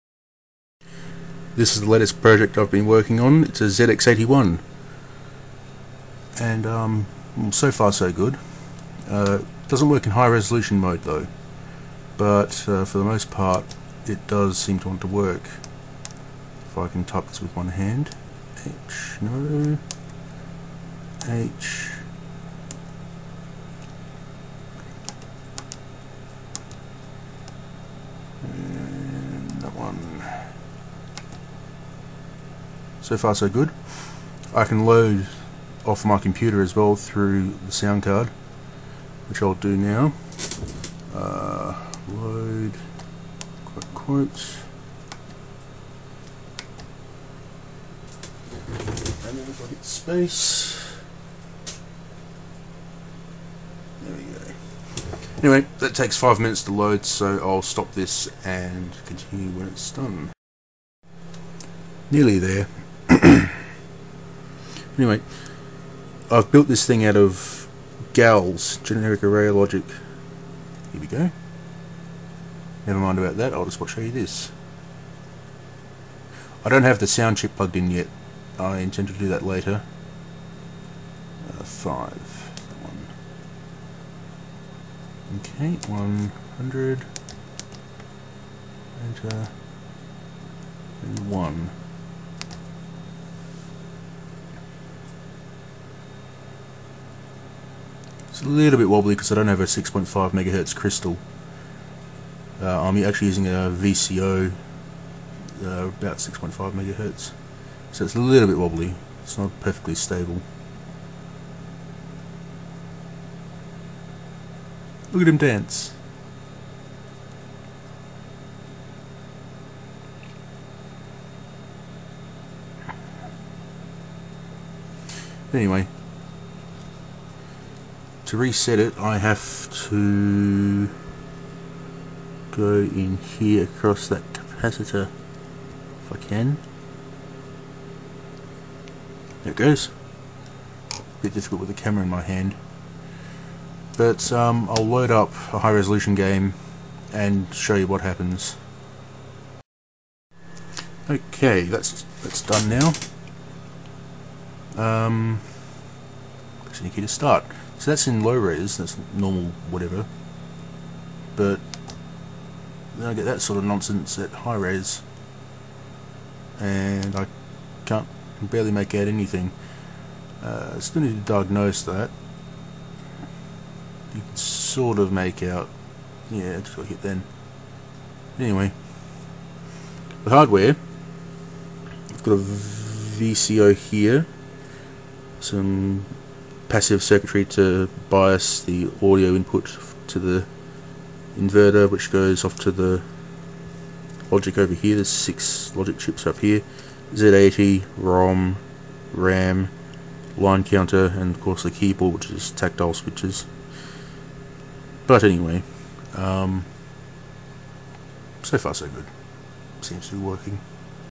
Homebrew ZX81 Computer
This is a Sinclair ZX81 implemented mostly in GAL IC's (six in total) and five 74HC IC's, plus the Z80, ROM and RAM. The chips and keyboard all fit onto the one board. There's enough room to add an AY-3-8912 sound chip, which I will do later.